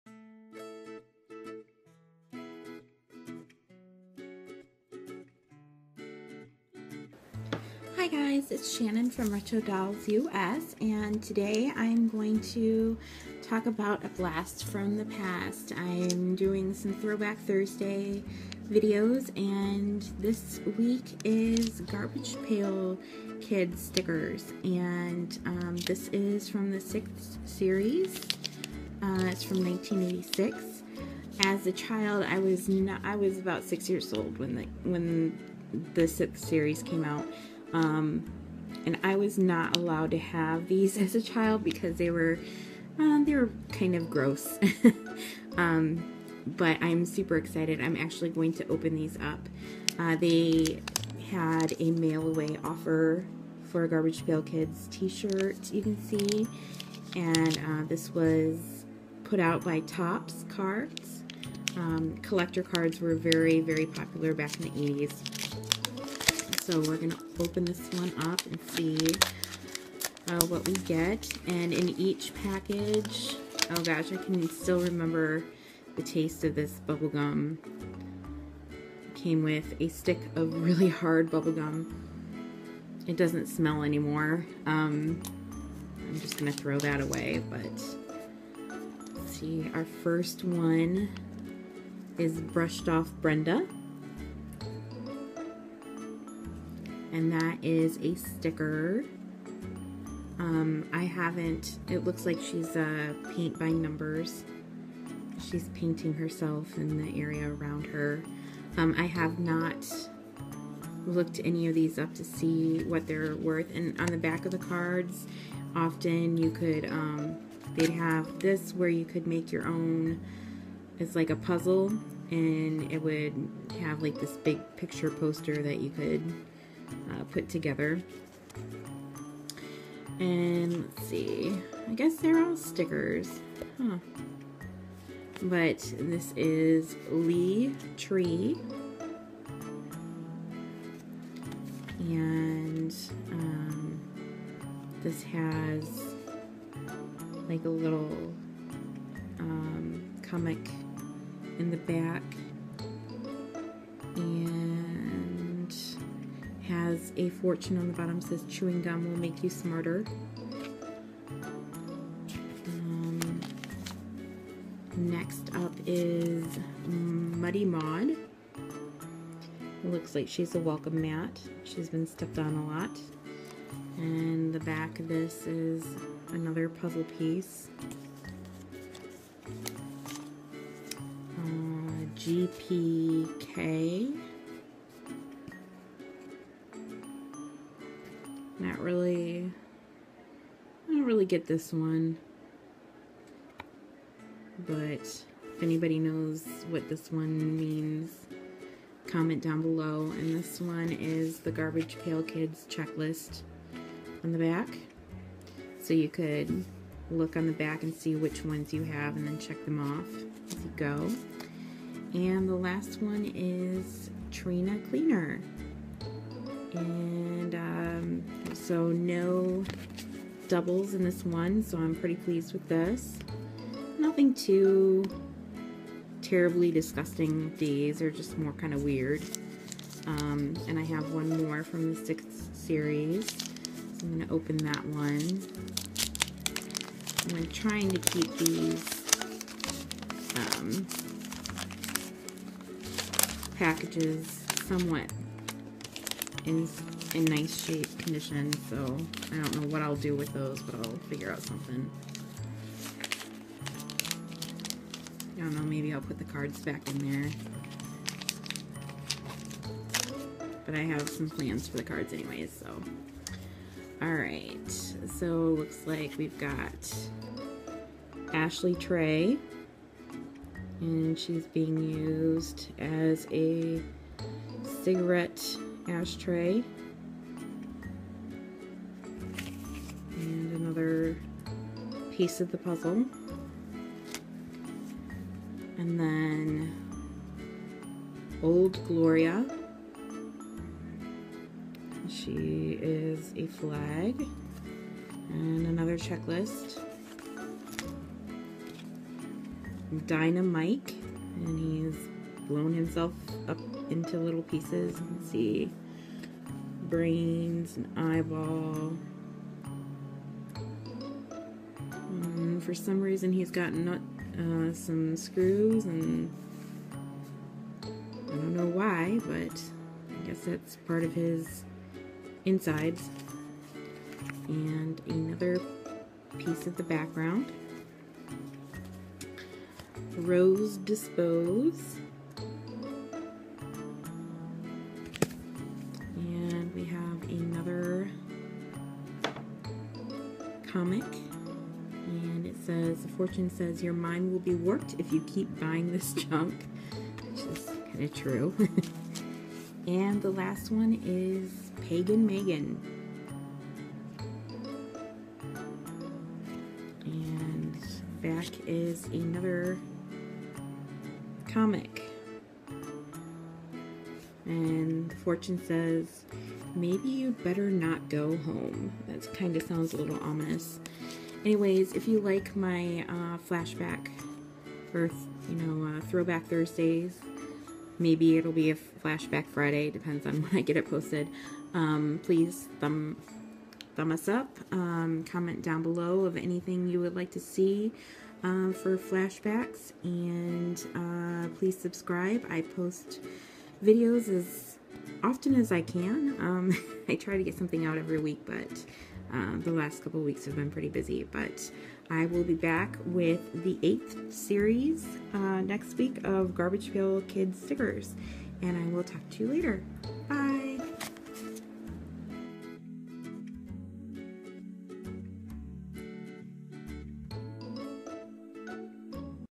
Throwback Thursday: Garbage Pail Kids Stickers 6th Series!
A few days late (I was out of town). New segment! Throwback Thursdays! This week we look at Garbage Pail Kids Stickers :)

Thanks Terrin for the link!! :)